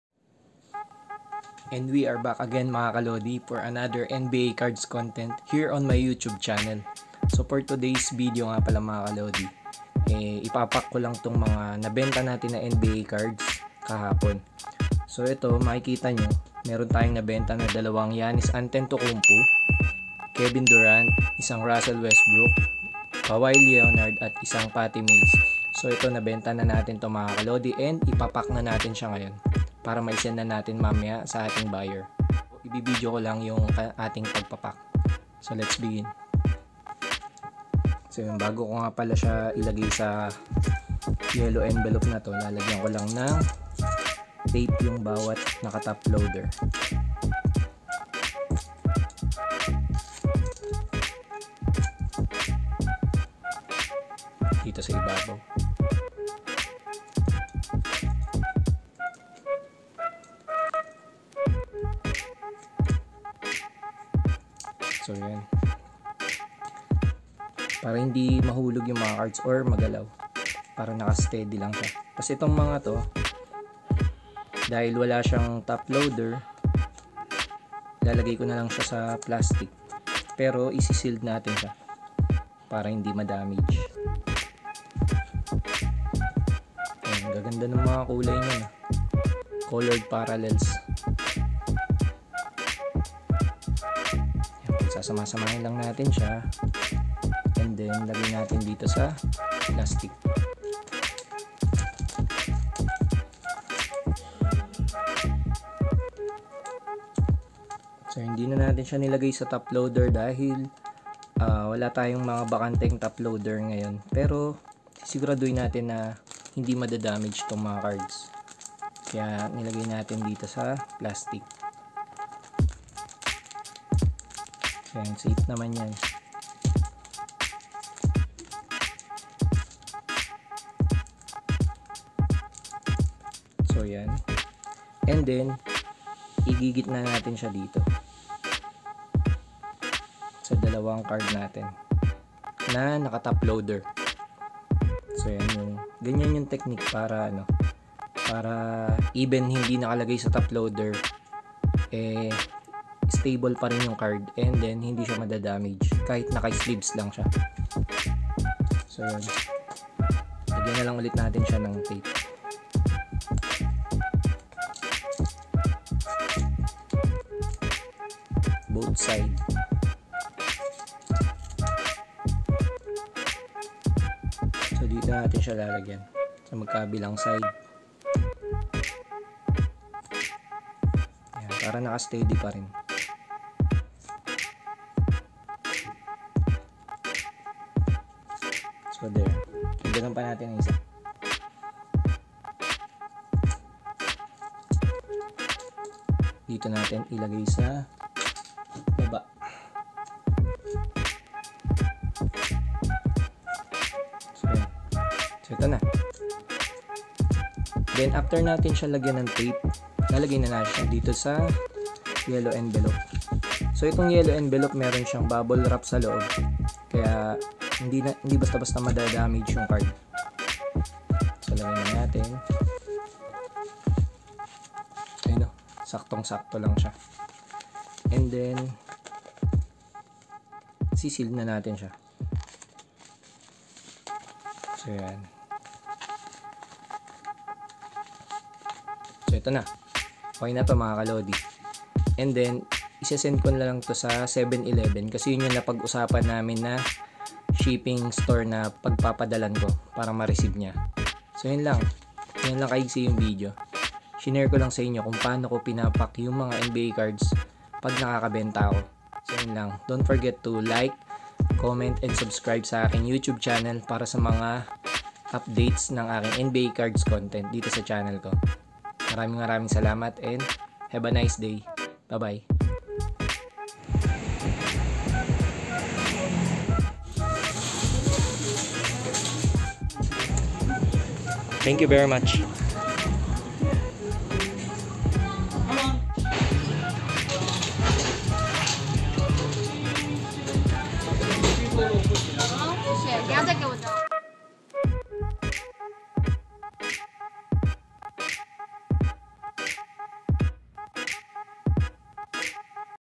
SOLD‼️For ONLY 140NTD‼️SUPER SULIT pero SOLID na CARDS | Nba Card Hobby / Hustle
Random SuperStar Cards Sold‼️

Super smooth deal to mga kaLodi dahil for ONLY 140NT or P250.00 pesos lang..

Apreciated you all guys for your Kind support to my Small Channel❤️

 Prizm Blaster Box Break #1

Prizm Blaster Box Break #2